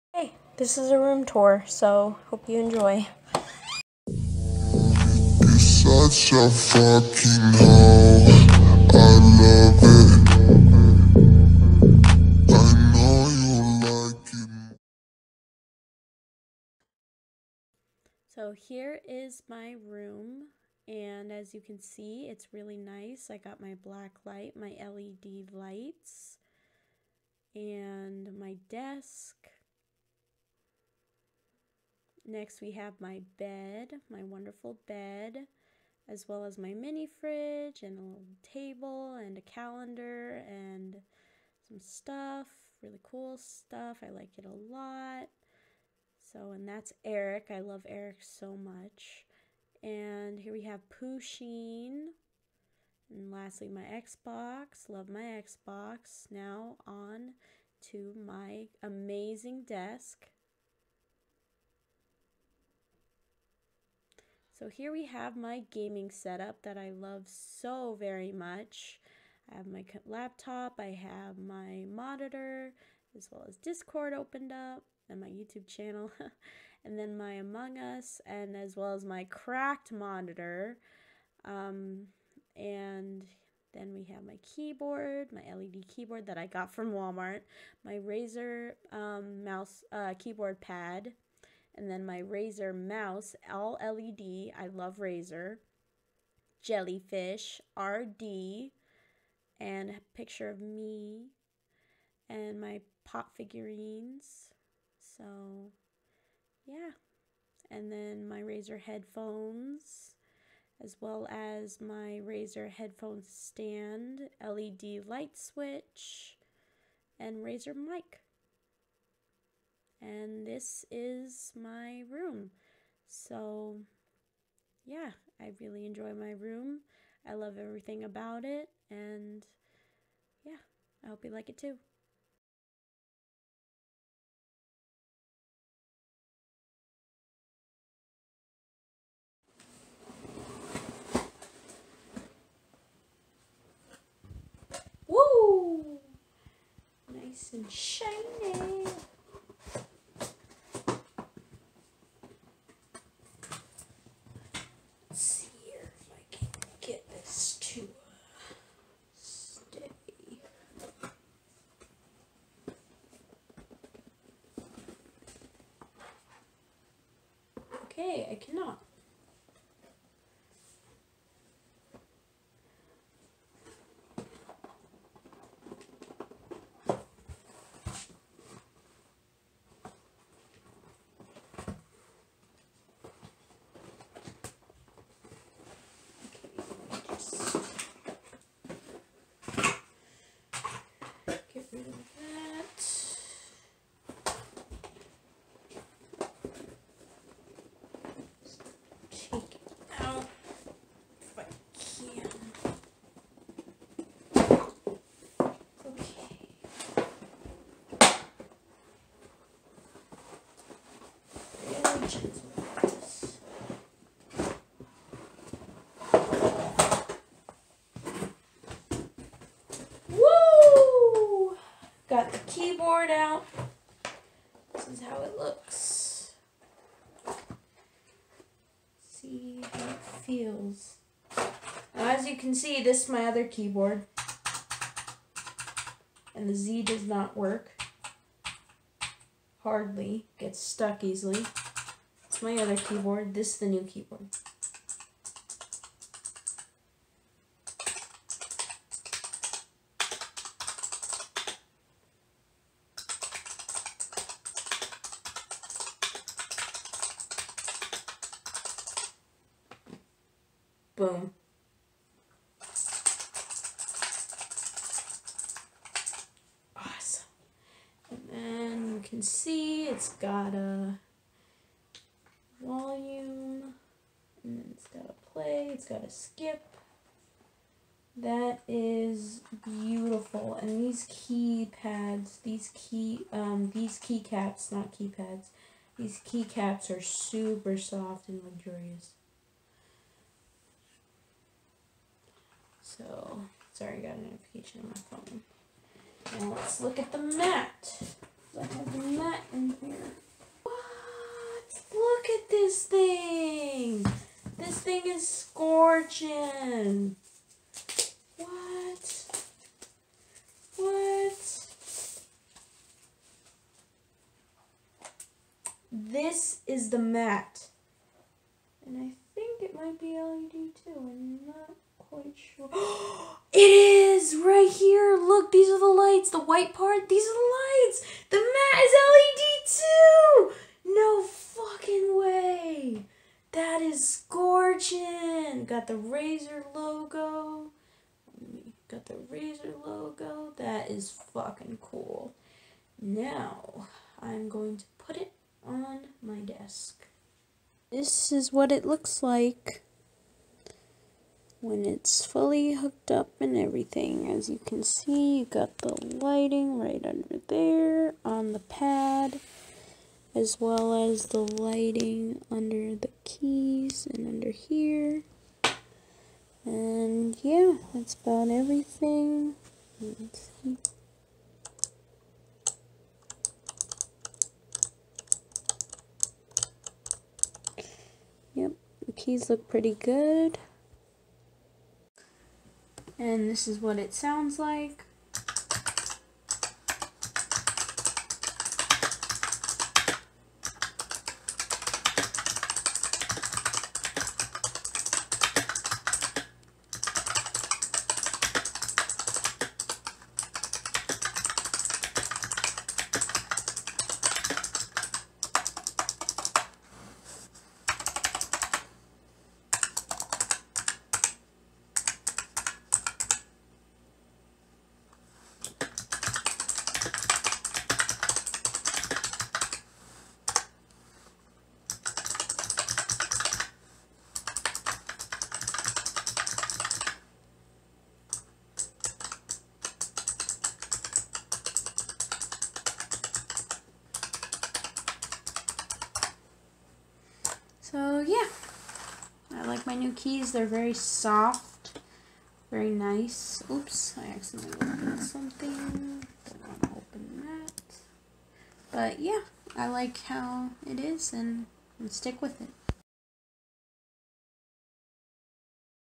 My 2021 Gaming Setup, Room Tour, AND Keyboard UNBOXING!?!?
▁ ▂ ▄ ▅ ▆ ▇ █ 𝓡𝓮𝓪𝓭 𝓜𝓸𝓻𝓮 █ ▇ ▆ ▅ ▄ ▂ ▁

So, when I recorded this, it was about 70% complete and all it needs is another monitor and the microphone stand. Right now, my microphone stand is being shipped and the monitor is gonna get removed this week! I'm SUPER excited and can't WAIT to see how it'll look! The Razer products were NOT sent to me for me to review and I bought them over the years with hard work! I just LOVE Razer and ALL of their products! To be exact, I have the Razer Keishi, Razer Phone 2, Razer Huntsman Elite, Razer Seiren, Razer Speakers, Razer Headset Stand, ]Razer Kraken Headset, Razer DeathAdder V2, AND Razer Goliathus! I try to buy ALL of your products! ALSO, if ya'll can PLEASE type in your FAVORITE Razer product and I hope you enjoyed this quick short video of my gaming setup and please leave a like and subscribe to the channel for more interesting videos! Bye guys!!